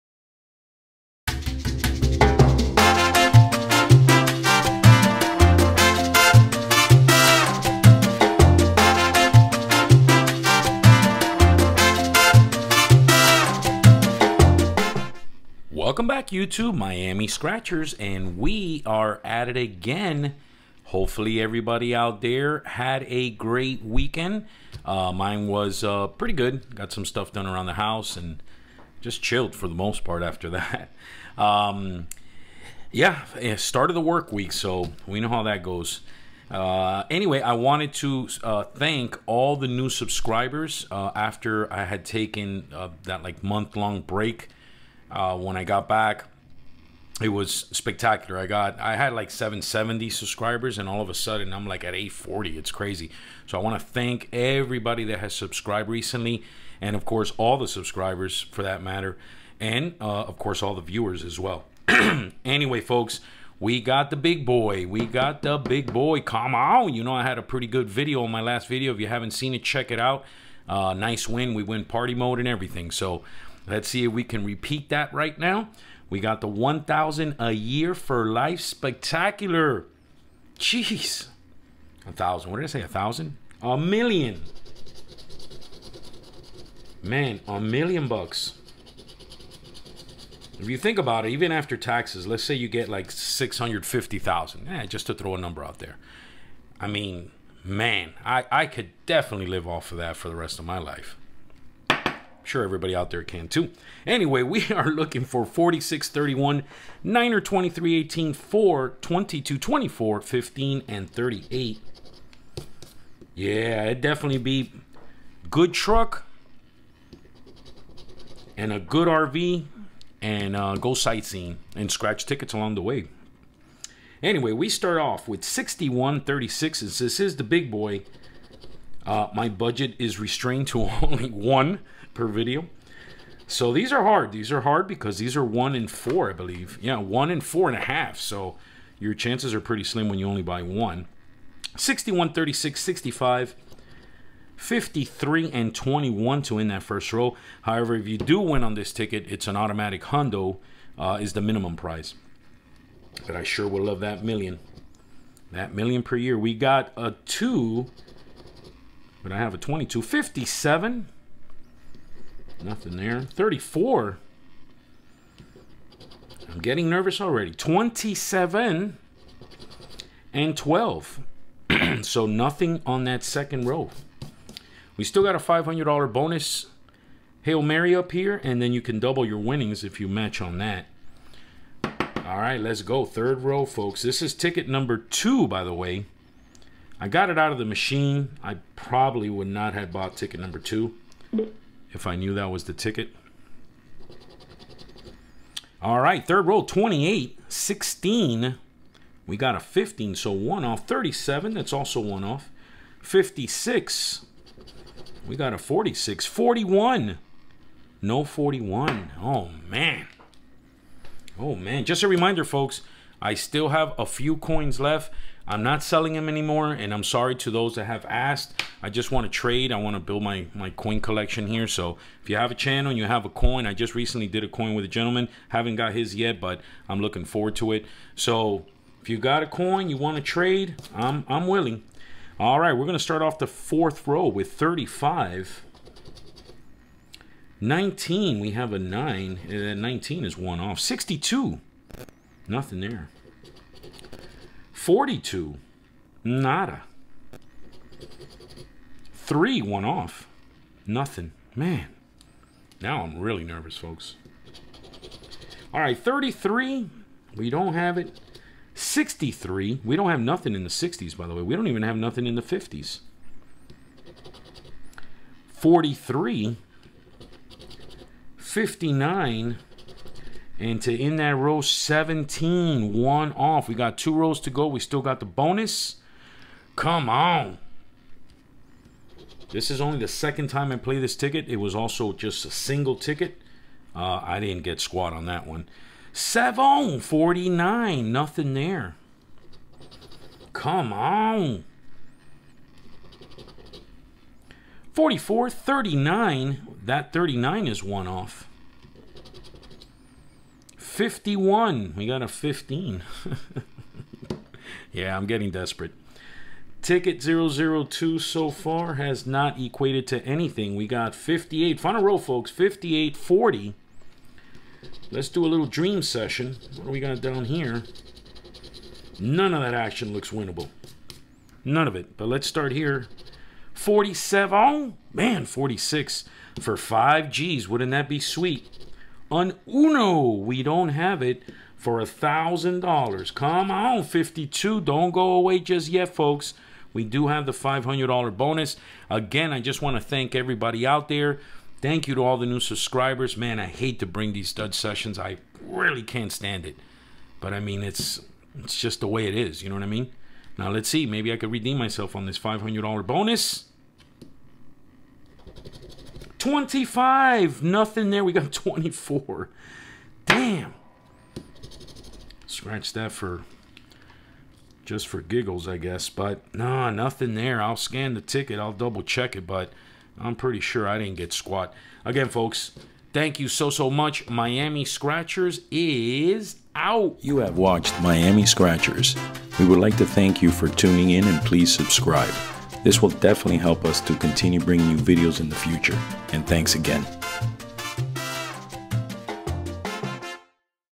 FLORIDA LOTTERY $50 $1,000,000 A YEAR FOR LIFE SPECTACULAR I GOT JUST ONE 😳
The New Florida Lottery $1,000,000 a year for life scratch off ticket.

We do not promote gambling and highly suggest if you do always do it in moderation. Know your limits and do not go over them.